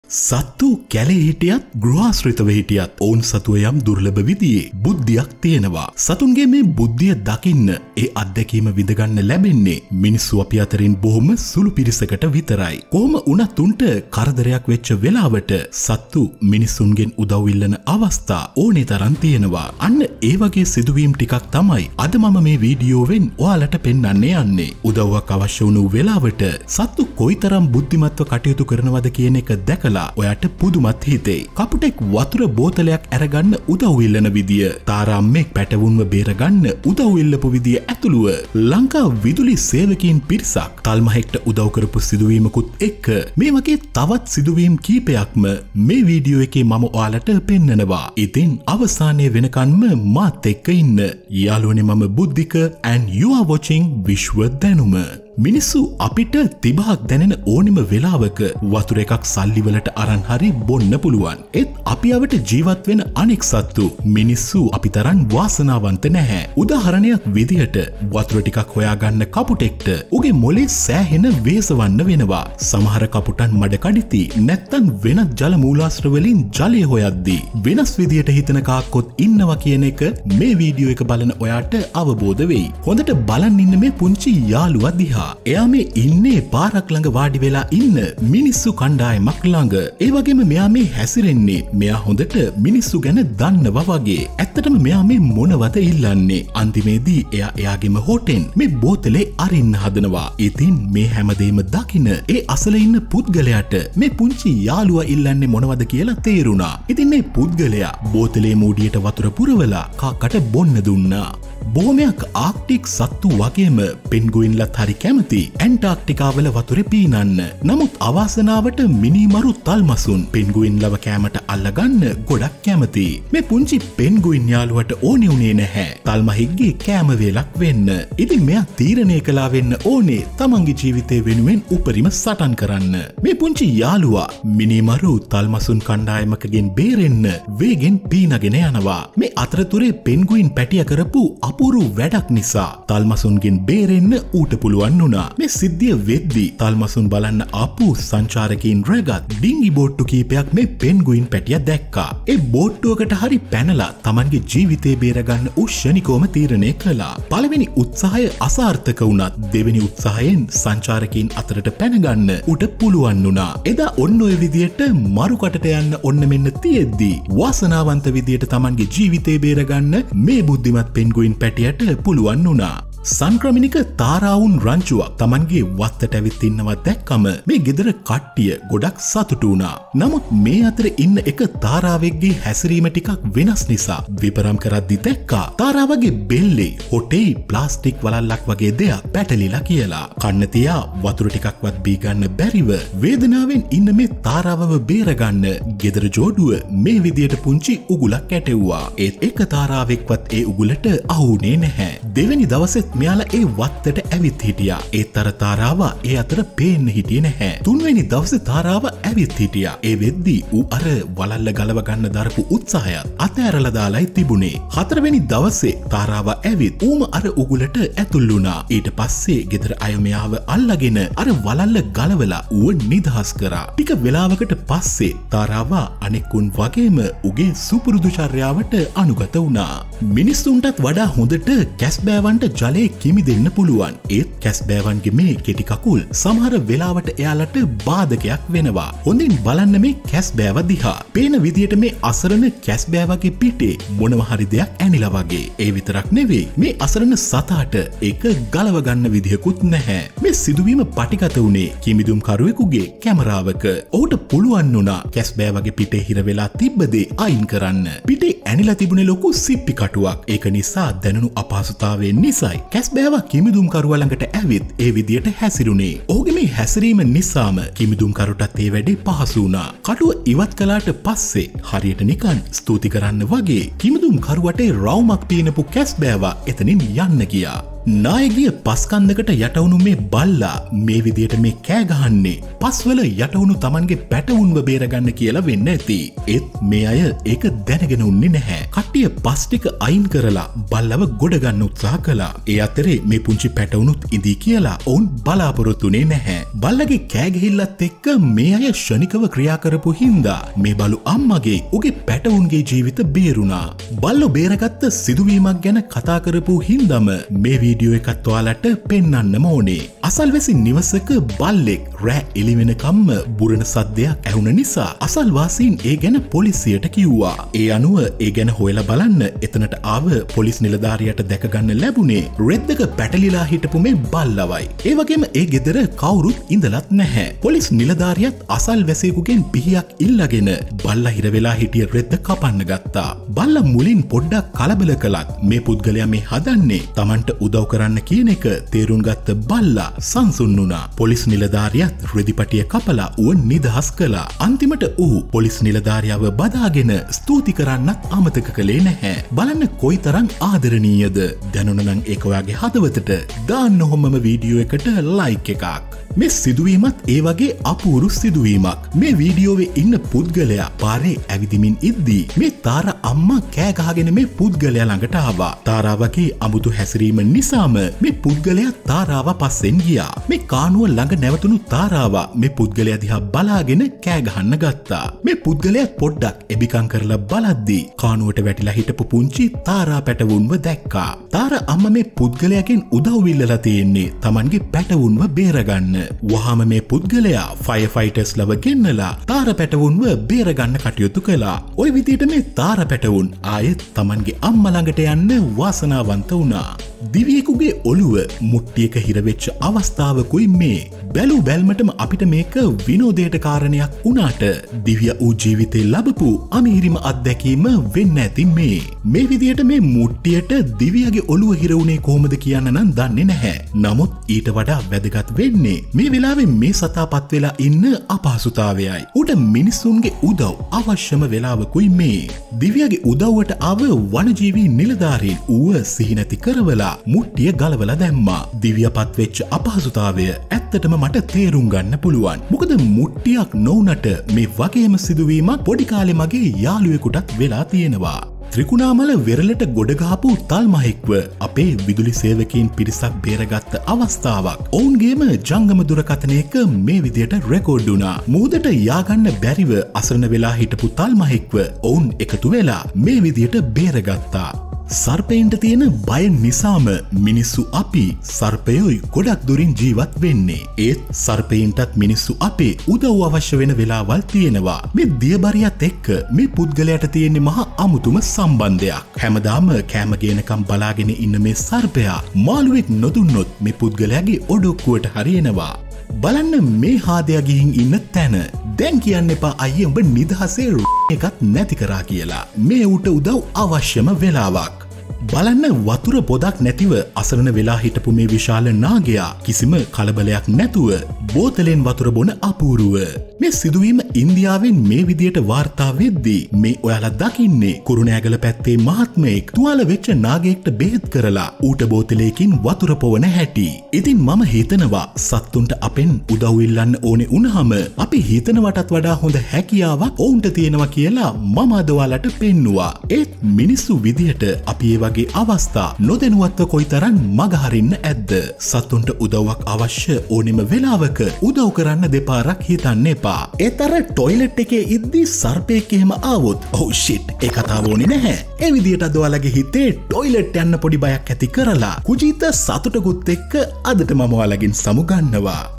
සත්තු මිනිසුන්ගෙන් උදවු ඉල්ලපු අවස්ථා | Animals That Asked People For Help (Part-01)
Animals have a rare intelligence, whether they live in the wild or domestic. Very few of us humans get to see and experience the intelligence of animals. However, when they are in trouble, there are plenty of times when animals need help from humans.

Today, I will show you a few such incidents in this video. You will be amazed after knowing how intelligent the animals are when they need help.